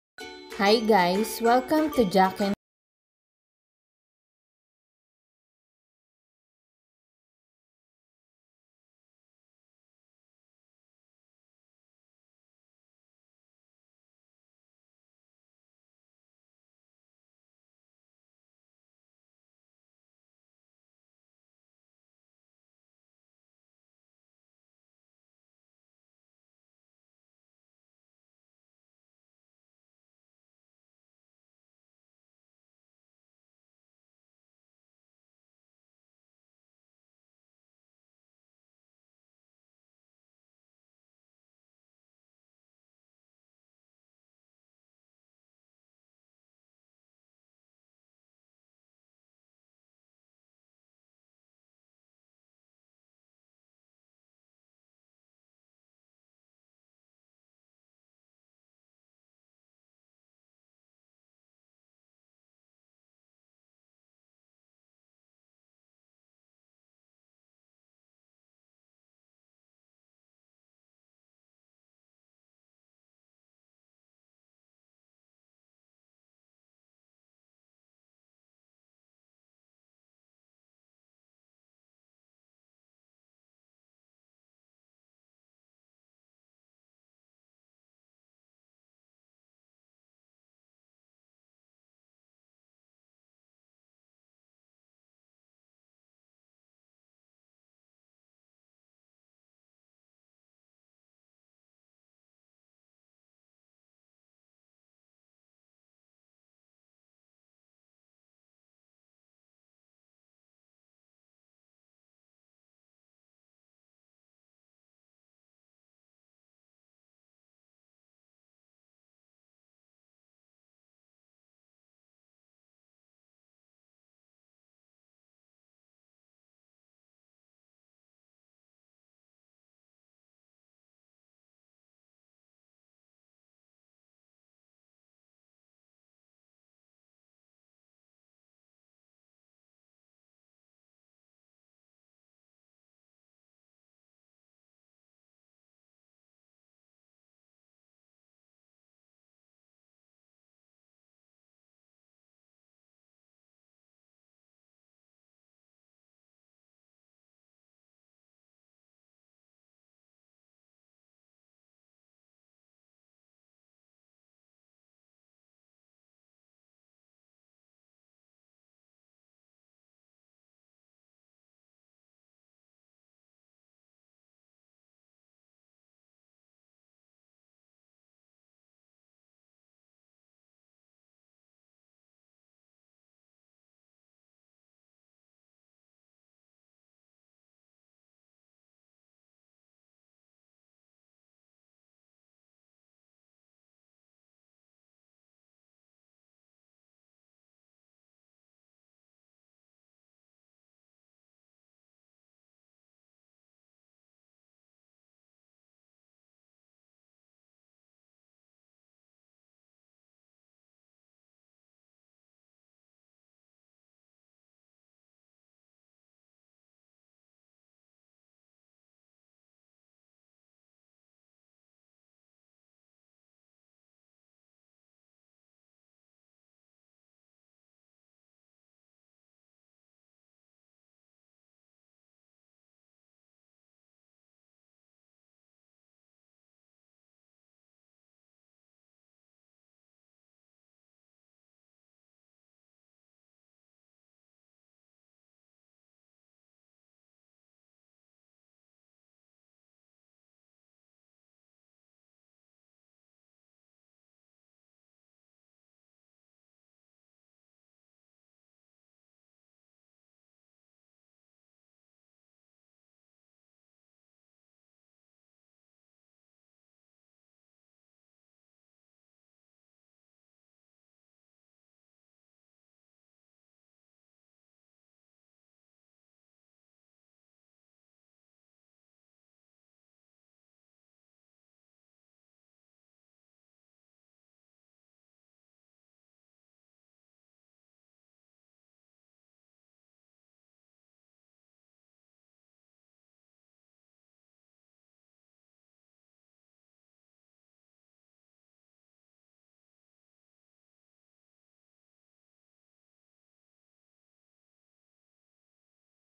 BEEF LO MEIN / EASY NOODLES RECIPE / Jack N Poy Kitchen
BEEF LO MEIN

INGREDIENTS:

363G LO MEIN NOODLES
1/4 KG BEEF (THIN SLICED)
OLIVE OIL
GROUND BLACK PEPPER
ONION SLICED
GARLIC CHOPPED
100G CARROTS JULIENNE
100G BELL PEPPER JULIENNE
YOUR CHOICE OF LEAFY GREENS
1/2 CUP BEEF STOCK
SPRING ONION CHOPPED

SAUCE MIXTURE:
1 TBSP SOY SAUCE
1 TBSP DARK SOY SAUCE
1 TBSP OYSTER SAUCE
1 TBSP CHINESE COOKING WINE
1 TBSP SESAME OIL
1 TSP CORNSTARCH
1 TBSP SUGAR